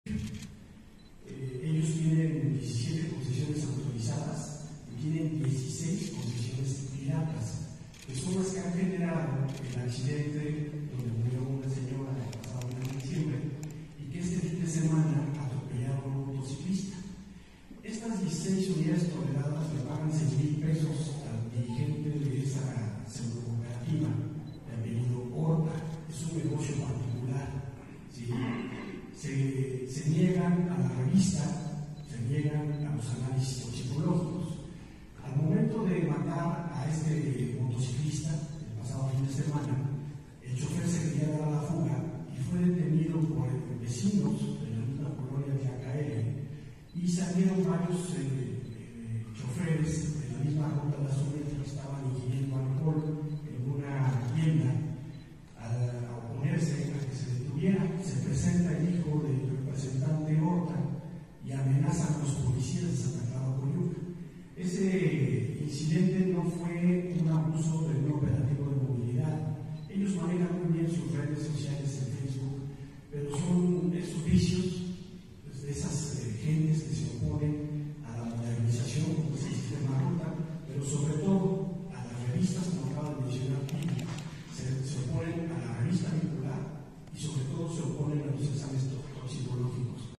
Gobierno de Puebla detecta 16 concesiones “piratas” en la Ruta Azumiatla
Omar Álvarez, secretario de Movilidad, informó que el chofer que fue detenido en Azumiatla este fin de semana intentó darse a la fuga después de atropellar a un motociclista. En esta junta auxiliar tienen identificadas 16 concesiones piratas cuyos conductores se oponen a pruebas toxicológicas y revista.